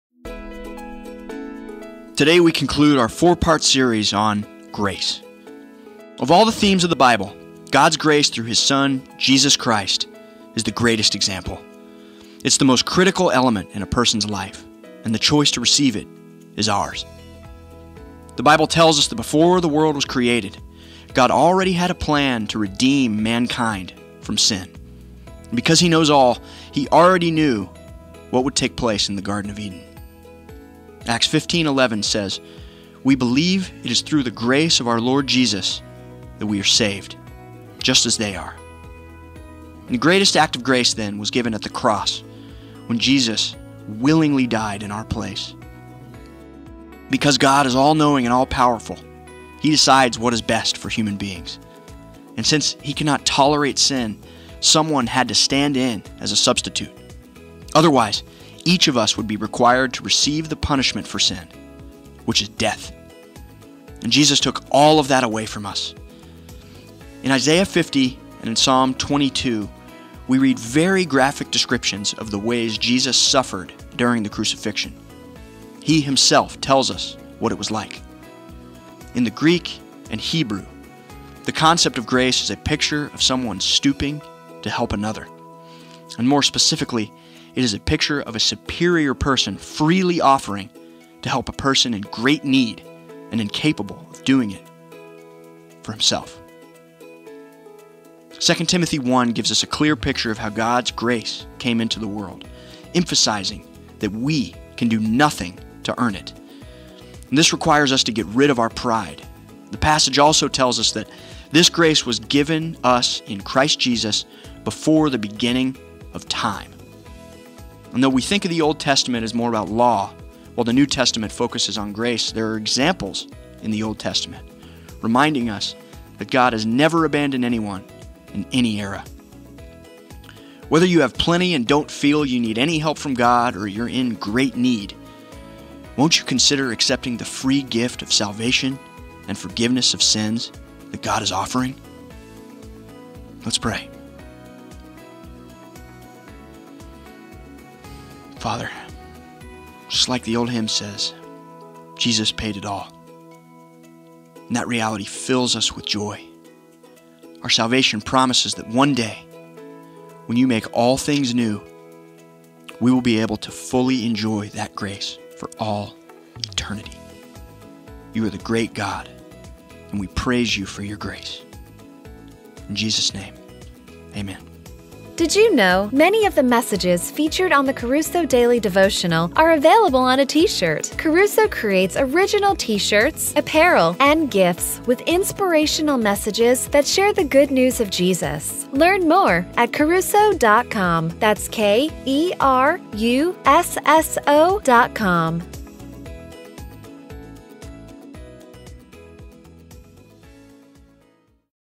Kerusso Daily Devotional - What is Grace? [4 of 4}: What is the Greatest Grace?
On Today’s episode of the Kerusso Daily Devotional, we talk about the highest ideal of grace, Jesus Christ.

Acts 15:11 says, “We believe it is through the grace of our Lord Jesus that we are saved, just as they are.”

All through Scripture, we see evidence of the work of our Redeemer, King Jesus. It is his grace and the cross that makes possible our reconciliation with God. He is gracious to us in various ways in this life, but salvation is an eternal gift.